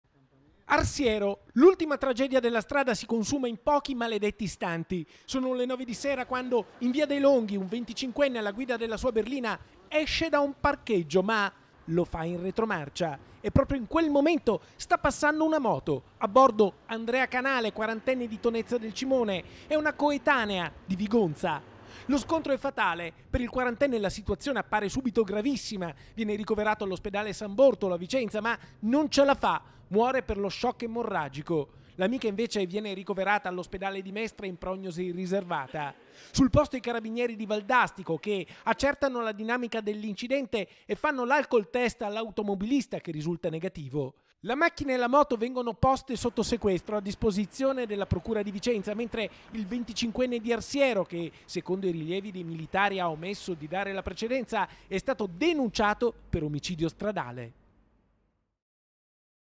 MORTO MOTOCICLISTA, VENTICINQUENNE DENUNCIATO PER OMICIDIO STRADALE | 26/08/2021
A3 NEWS Veneto 26/08/2021 - ARSIERO - Un motociclista è morto in ospedale al San Bortolo. Si era scontrato ieri sera con un'auto in retromarcia.  || Arsiero. L'ultima tragedia della strada si consuma in pochi, maledetti istanti. Sono le nove di sera quando in Via Dei Longhi un venticinquenne, alla guida della sua berlina esce da un parcheggio. Ma lo fa in retromarcia e proprio in quel momento sta passando una moto: a bordo Andrea Canale, 40enne di Tonezza del Cimone, e una coetanea di Vigonza. Lo scontro è fatale: per il quarantenne la situazione appare subito gravissima: viene ricoverato all'ospedale San Bortolo, a Vicenza, ma non ce la fa: muore per lo shock emorragico. L'amica invece viene riocoverata all'ospedale di Mestre in prognosi riservata. Sul posto i Carabinieri di Valdastico che accertano la dinamica dell'incidente e fanno l'alcoltest all'automobilista, che risulta negativo. La macchina e la moto vengono poste sotto sequestro, a disposizione della Procura di Vicenza, mentre il venticinquenne di Arsiero, che secondo i rilievi dei militari ha omesso di dare la precedenza, è stato denunciato per omicidio stradale. (Servizio di Carlo Alberto Inghilleri) - Segui Antenna Tre anche sul digitale terrestre! Visita il sito